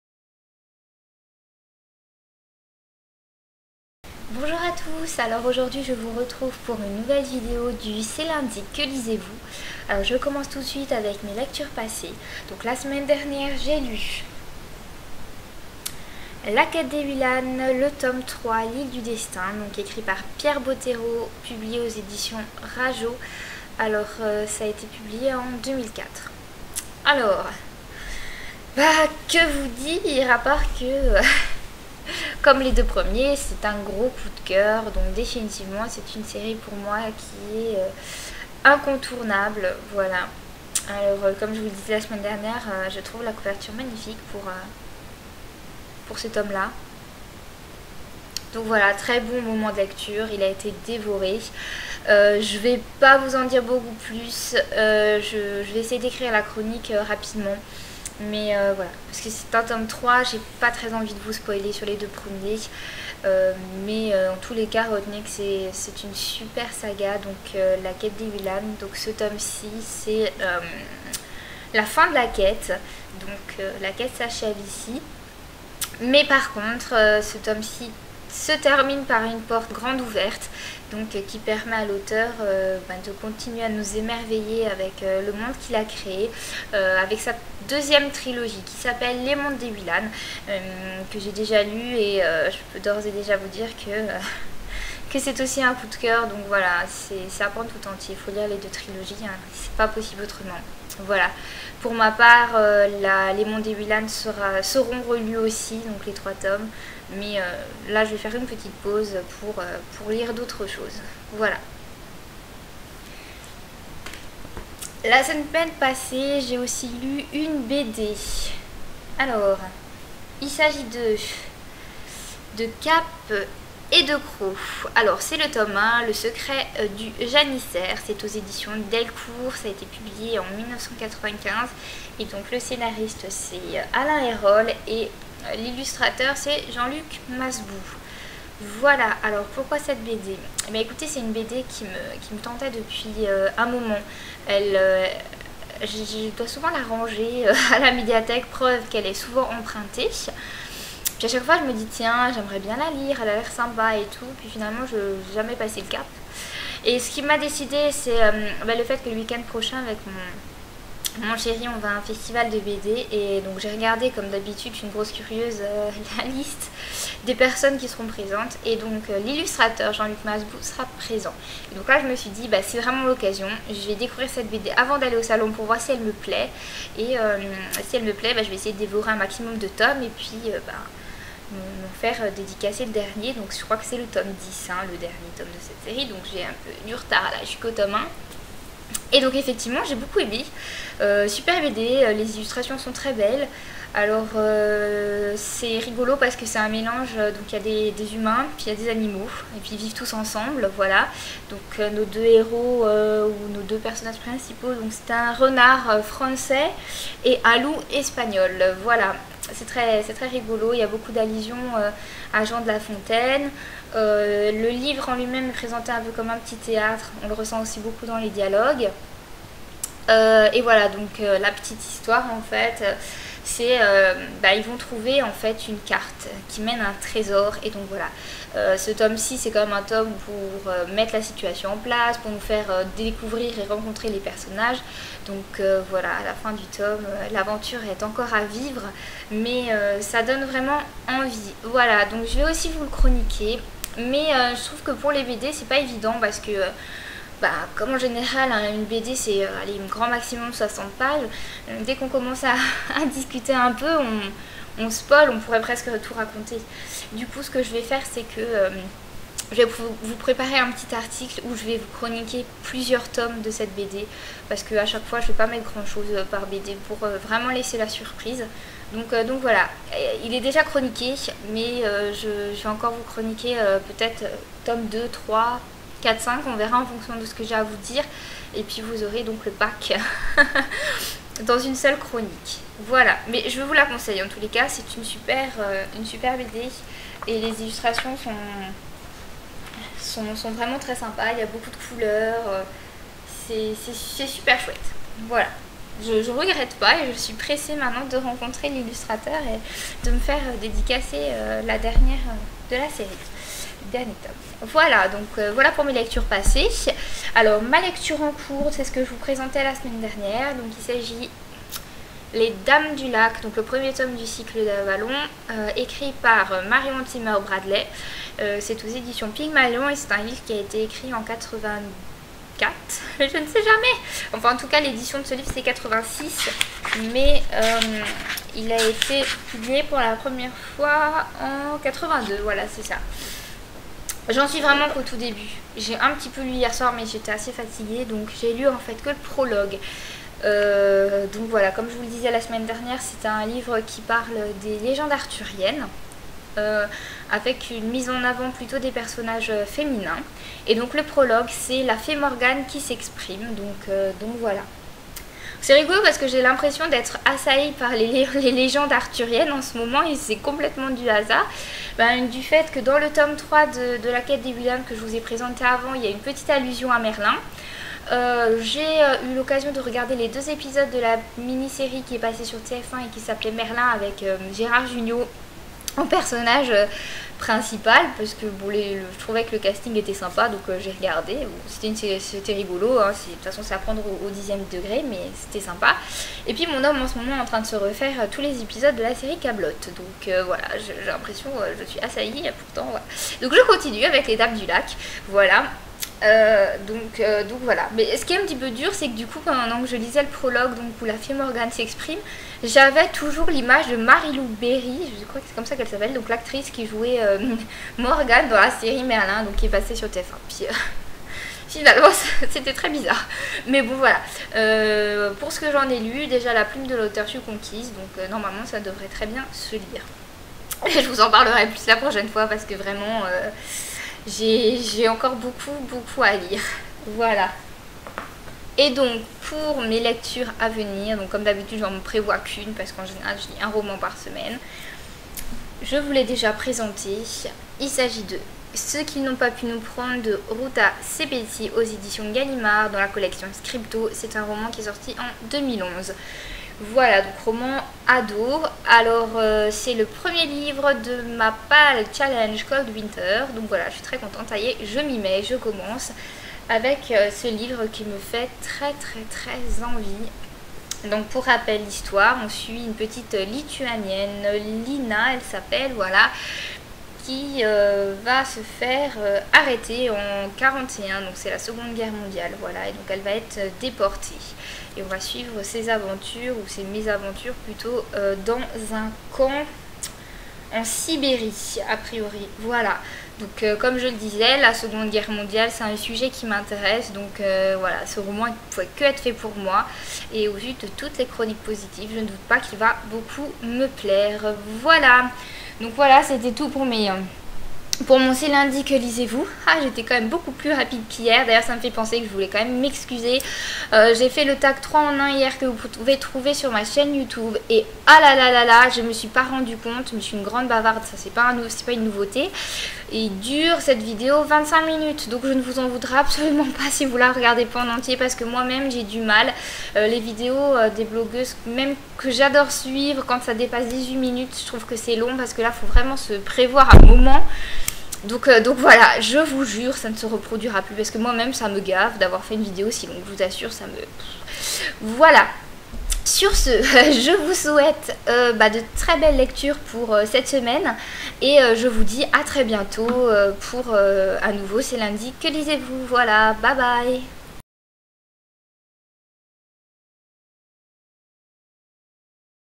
C'est Lundi, que lisez-vous? #5 (12/11/2012)
Rendez-vous de la blogosphère instauré en français par Mallou et ensuite repris par Galleane.

Mes lectures passées :
• "La Quête d'Ewilan (t.3) : L'Île du destin" / Pierre Bottero. Editions Rageot. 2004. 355 p.
• "De cape et de crocs (t.1) : Le Secret du janissaire" / Alain Ayroles (scénario) & Jean-Luc Masbou (dessin). Editions Delcourt - Coll. Terres de Légendes. 1995. 46 p.
Ma lecture en cours :
• "Les Dames du lac" (t.1) / Marion Zimmer Bradley. Editions Pygmalion. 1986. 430 p.
Ma lecture à venir :
• "Ce qu'ils n'ont pas pu nous prendre / Ruta Sepetys. Editions Gallimard - Coll. Scripto. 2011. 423 p.